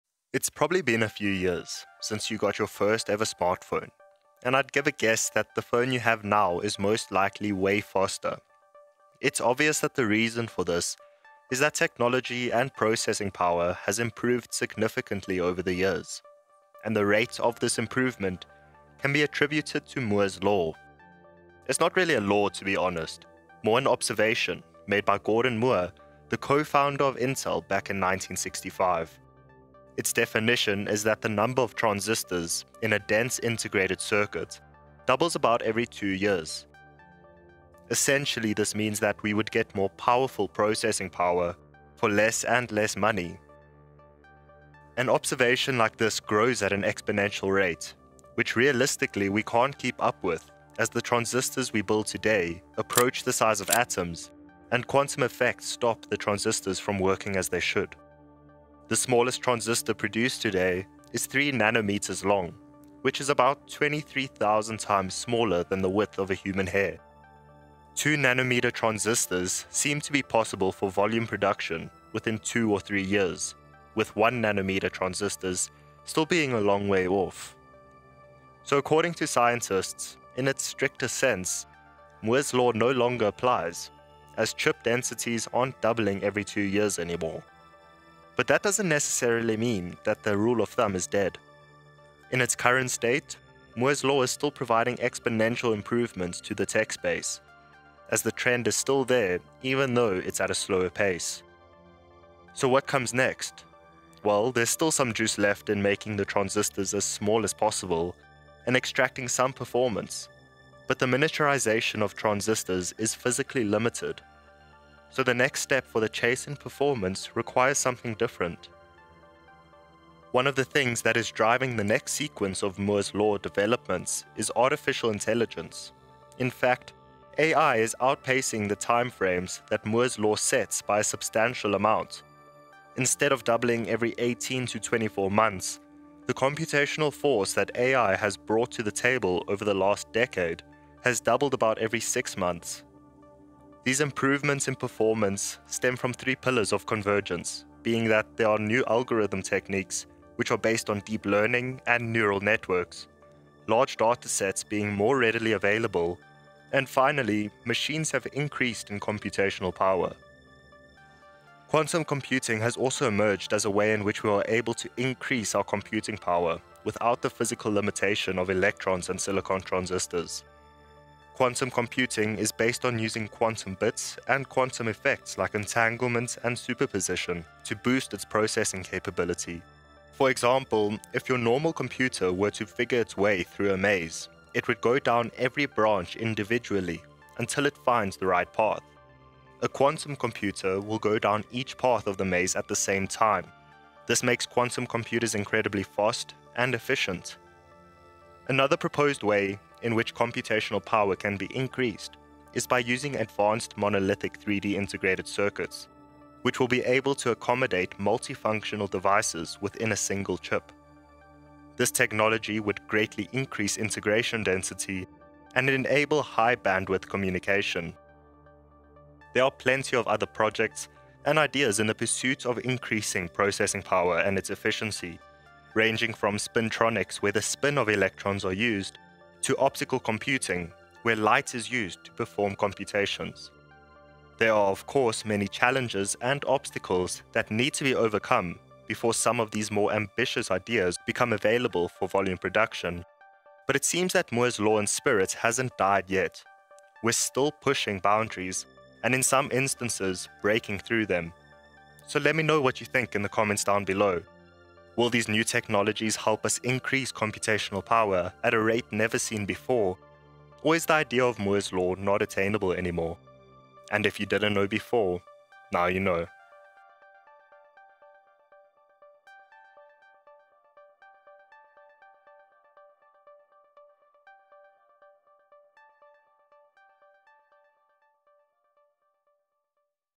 More Than Moore's Law - What's Next?
Moore's Law has been the rule of thumb that has propelled innovation in computing power of the last few decades. We have however finally reached a point where Moore's Law no longer strictly applies but its idea of innovation still carries on through other channels of chasing computing power.

This video discusses the end of Moore's Law and which exciting innovations will carry on its principle into the future.

Contents of the video:

0:00 – What is Moore's Law?
0:44 – We can't keep up with Moore's Law
2:00 – Artificial Intelligence
2:41 – Quantum Computing
3:18– 3D Integrated Circuits
3:39 – The future of Moore's Law

Enjoyed this video? Watch the previous one here!